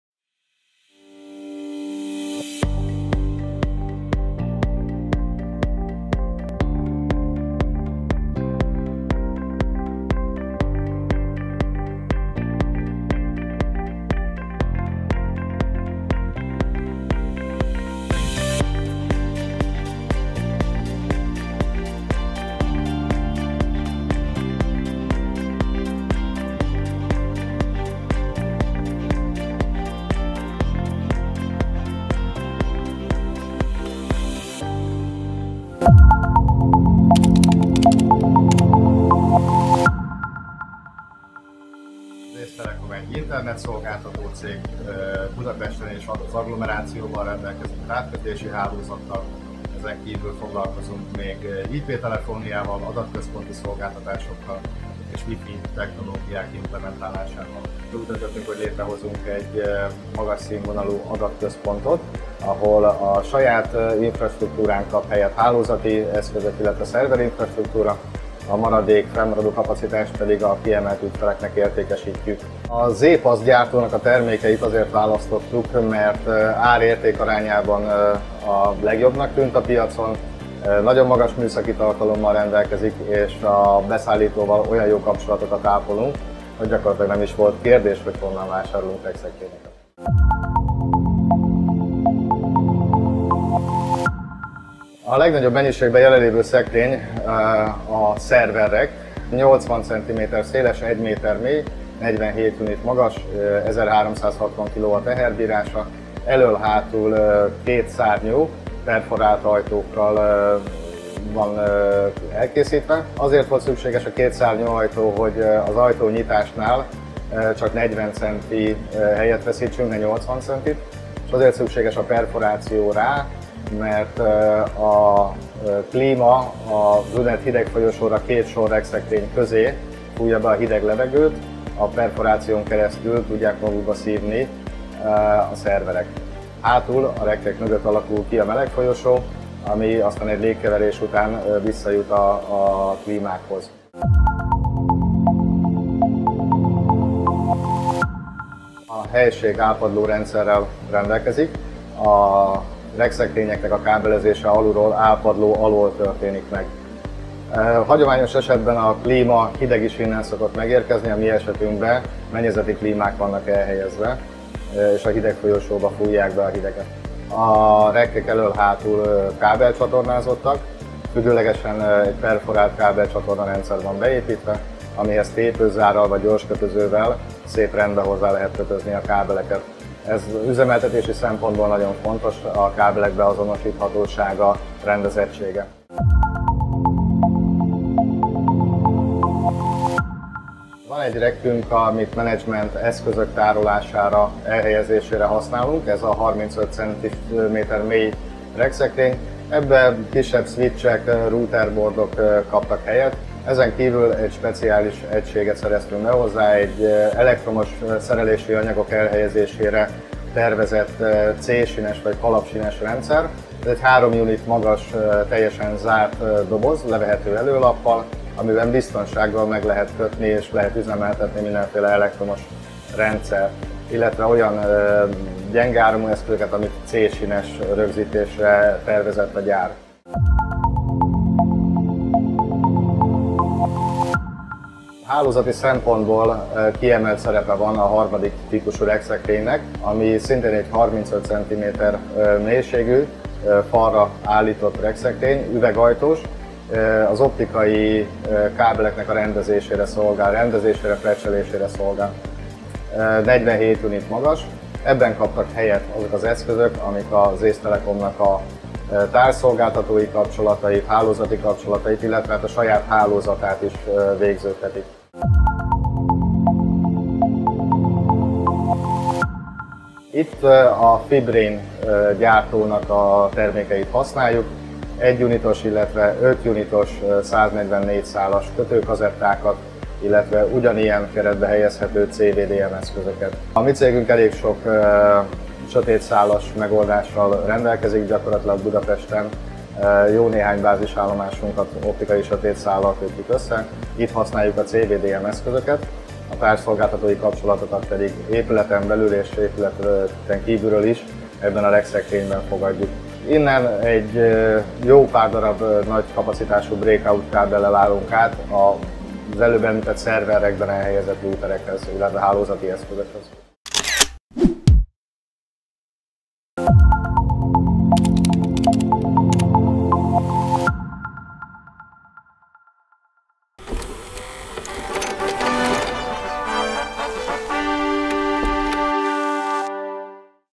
ZPAS Datacenter megoldások az ACE Telecom-nál - ENGLISH SUBTITLES
Videónkból kiderül, hogyan alkalmazta az ACE Telecom a ZPAS adatközponti megoldásait kiegészítve a Fibrain passzív CWDM rendszerével.